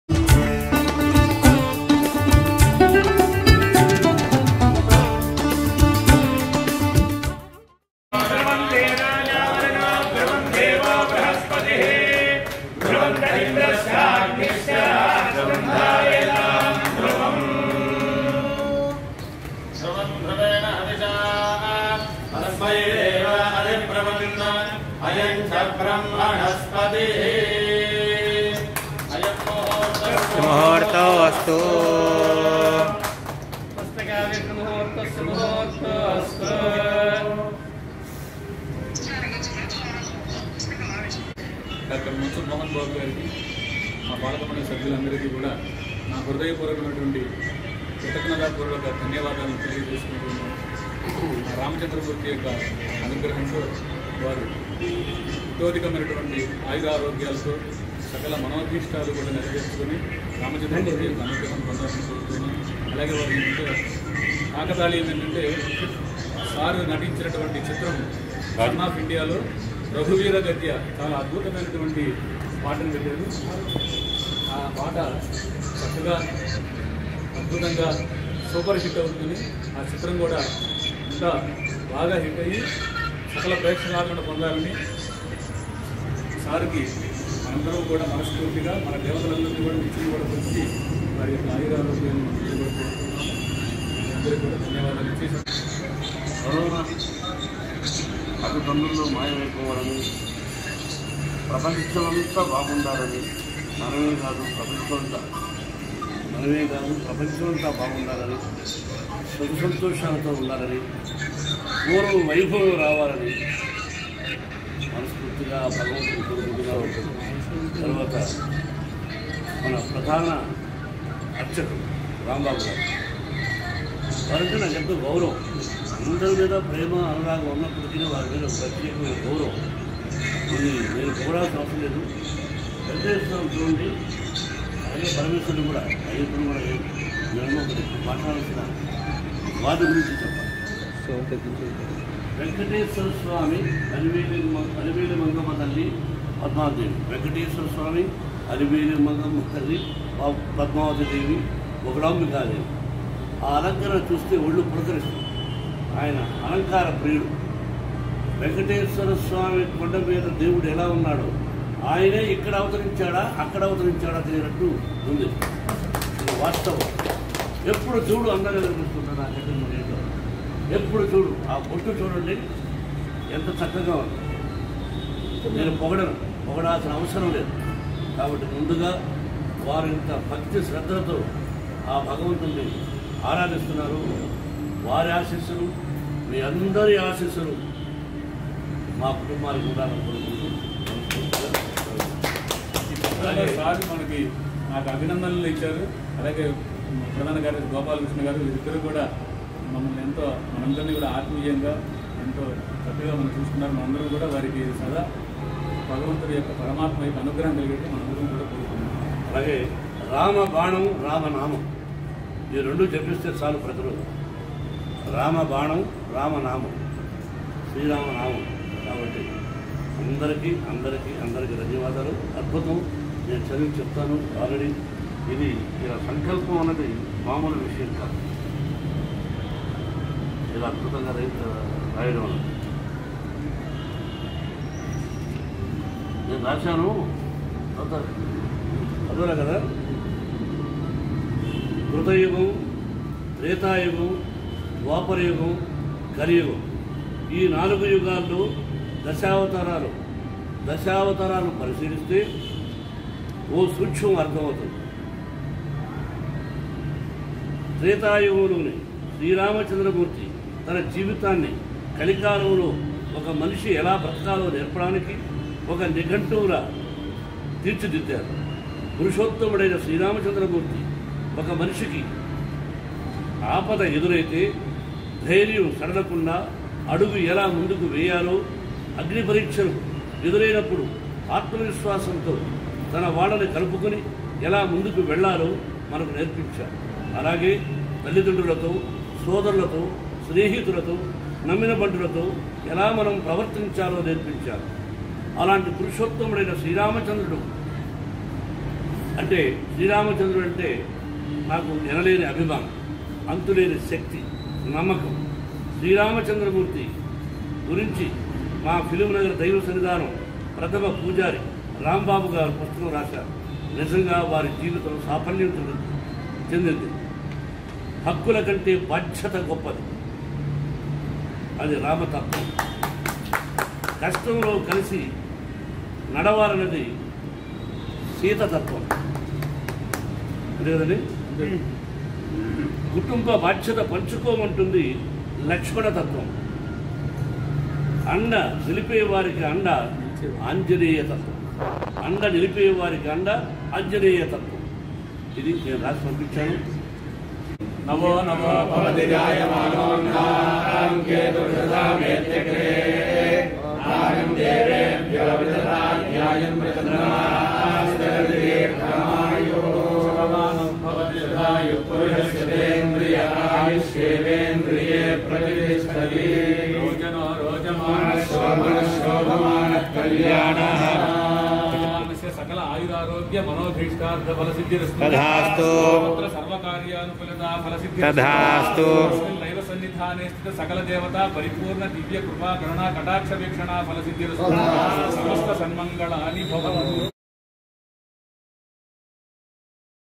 Mohan Babu Released "Rama Banam" Book | Shaili & shaili TV
Film Ngar Daivasannidhanam Chairman M. Mohan Babu Released "Rama Banam " Book. Watch Video. @Shaili & Shaili TV